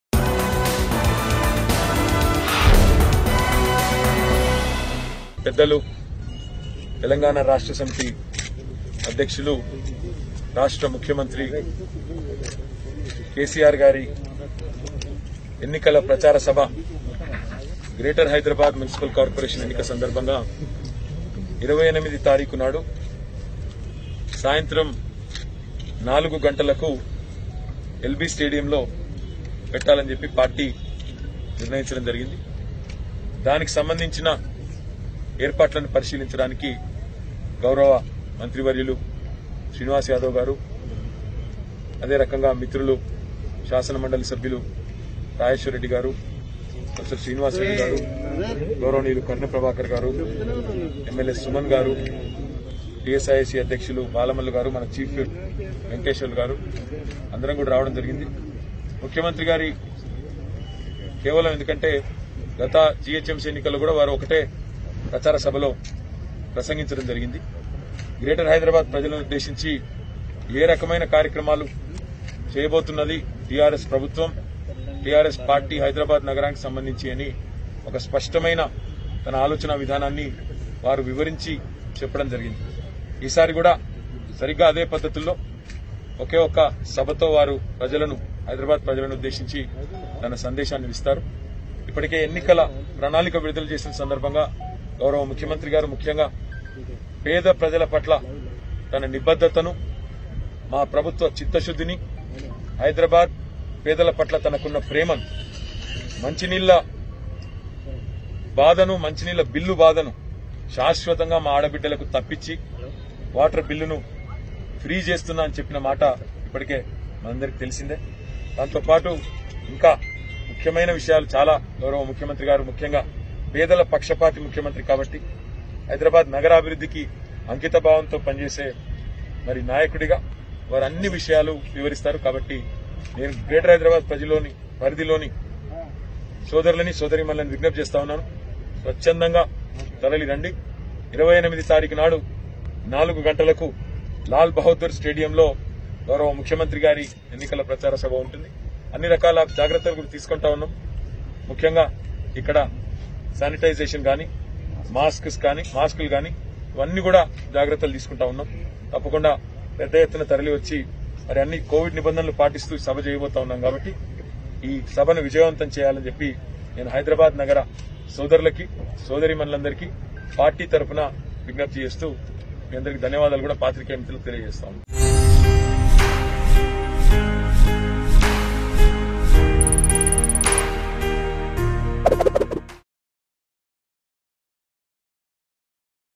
Minister KTR Speaks About CM KCR Campaigning In GHMC Elections 2020 | Telangana News | Mango News
Minister KTR Speaks About CM KCR Campaigning In GHMC Elections 2020.